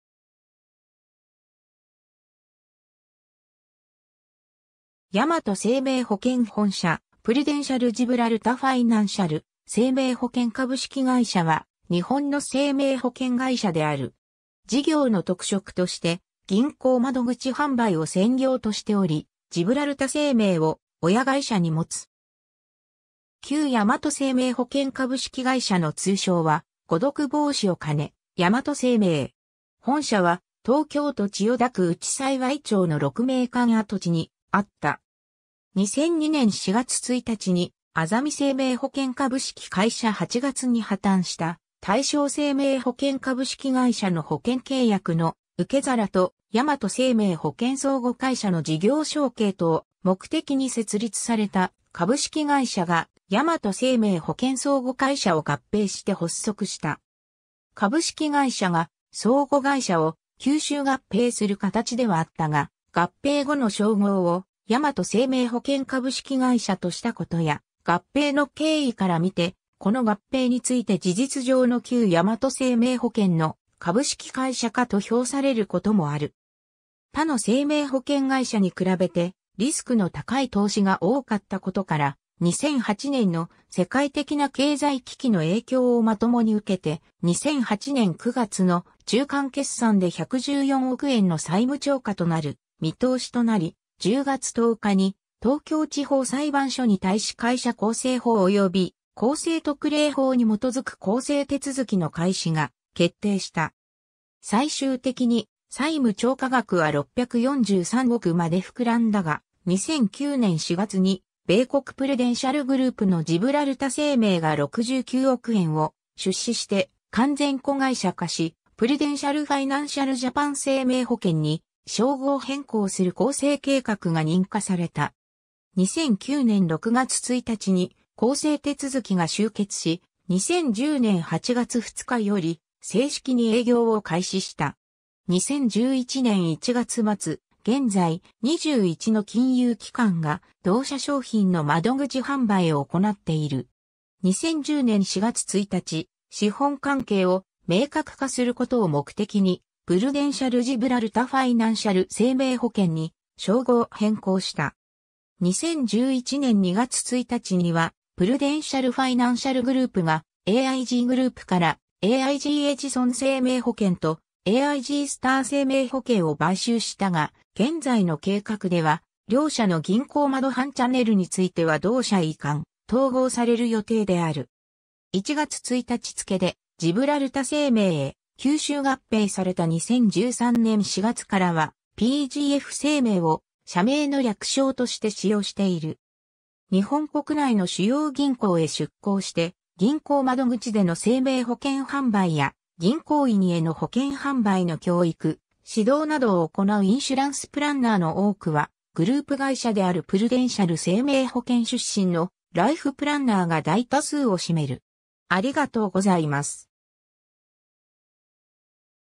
プルデンシャル ジブラルタ ファイナンシャル生命保険
大和生命保険本社（現 日比谷U-1ビル） プルデンシャルジブラルタファイナンシャル生命保険株式会社（プルデンシャルジブラルタファイナンシャルせいめいほけん、英称：The Prudential Gibraltar Financial Life Insurance Co., Ltd.）は、日本の生命保険会社である。
 事業の特色として銀行窓口販売(バンカシュランス)を専業としており、ジブラルタ生命を親会社に持つ。
 旧大和生命保険株式会社の通称は誤読防止を兼ね「ヤマト生命」。
本社は、東京都千代田区内幸町の鹿鳴館跡地にあった。
 2002年（平成14年）4月1日に、あざみ生命保険株式会社（2000年（平成12年）8月に破綻した大正生命保険株式会社の保険契約の受け皿と、大和生命保険相互会社の事業承継とを目的に設立された株式会社）が、大和生命保険相互会社（以下、旧大和生命保険）を合併して発足した。
株式会社が相互会社を吸収合併する形ではあったが、合併後の商号を「大和生命保険株式会社」としたことや、合併の経緯から見て、この合併について事実上の旧大和生命保険の株式会社化と評されることもある。
 他の生命保険会社に比べて、リスクの高い投資が多かったことから、2008年（平成20年）の世界的な経済危機の影響をまともに受けて、2008年9月の中間決算で114億円の債務超過となる見通しとなり、10月10日に東京地方裁判所に対し会社更生法及び更生特例法に基づく更生手続の開始が決定した。
最終的に債務超過額は643億まで膨らんだが、2009年4月に、米国プルデンシャル・グループのジブラルタ生命が69億円を出資して完全子会社化し、プルデンシャル ファイナンシャル ジャパン生命保険に商号変更する更生計画が認可された。
 2009年6月1日に更生手続が終結し、2010年8月2日より正式に営業を開始した。
2011年1月末現在、21の金融機関（銀行）が同社商品の窓口販売を行っている。
 2010年（平成22年）4月1日、資本関係を明確化することを目的にプルデンシャルジブラルタファイナンシャル生命保険に商号変更した。
 2011年（平成23年）2月1日にはプルデンシャル・ファイナンシャルグループが、AIGグループからAIGエジソン生命保険とAIGスター生命保険を買収したが、現在の計画では両社の銀行窓販チャネルについては同社へ移管・統合される予定である。
（なおエジソン生命とスター生命は、2012年（平成24年）1月1日付でジブラルタ生命へ吸収合併された） 2013年4月からは、「PGF生命」を社名の略称として使用している。
 日本国内の主要銀行へ出向して、銀行窓口での生命保険販売や、銀行員への保険販売の教育・指導などを行うインシュランスプランナー(IP)の多くは、グループ会社であるプルデンシャル生命保険出身のライフプランナーが大多数を占める。